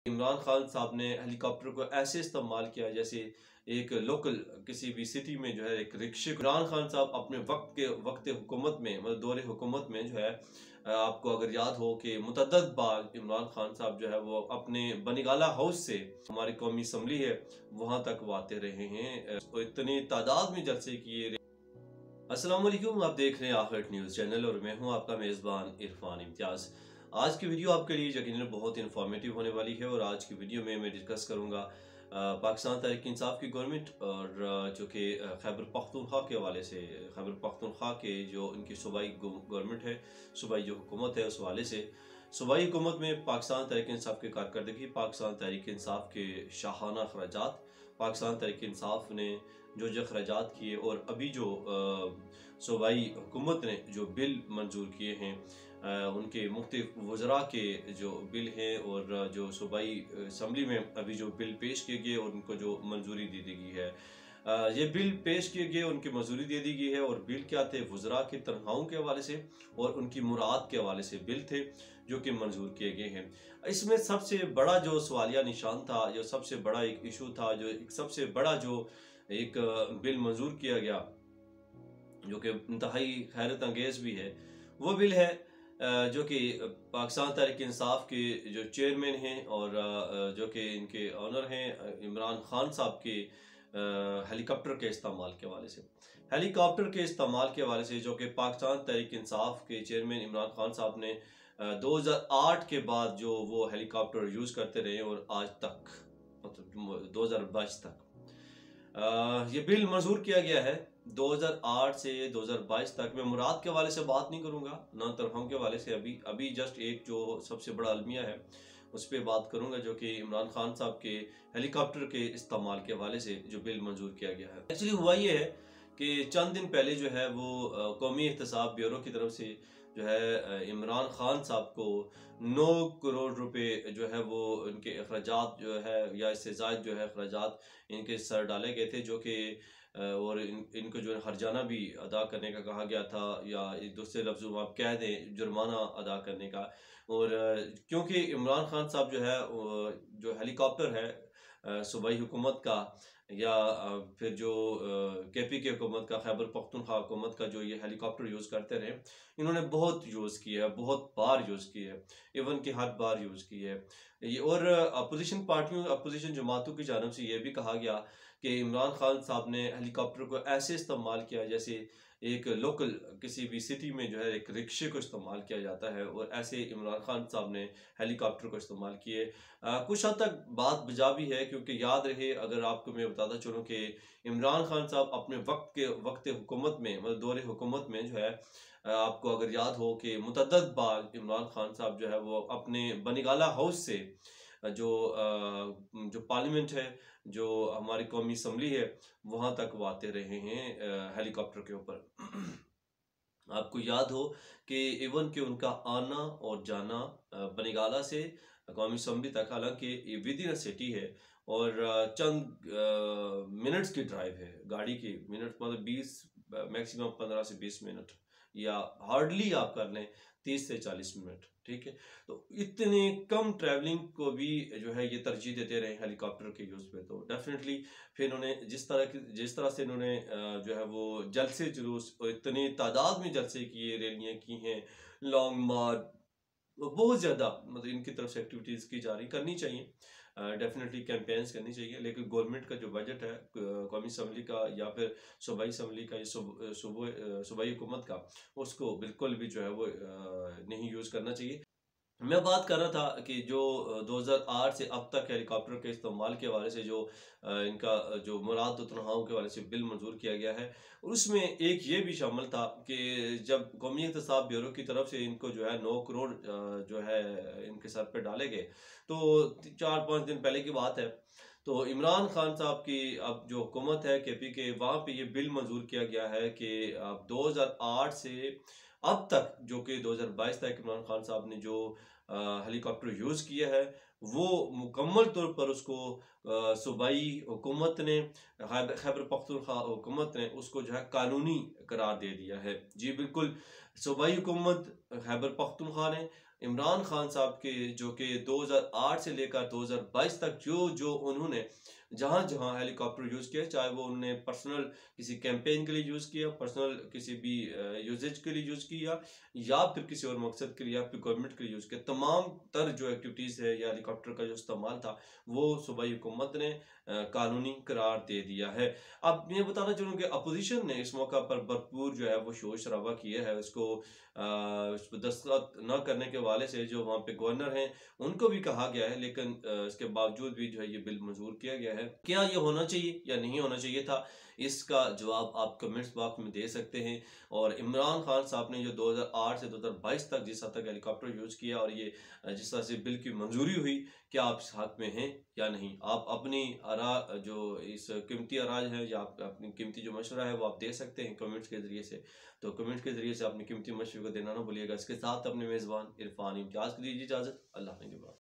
KPK budget 2022 23|Imran khan helicopter usage decision|Irfan Imtiaz
KPK budget 2022 23|Imran khan helicopter usage decision|Kpk government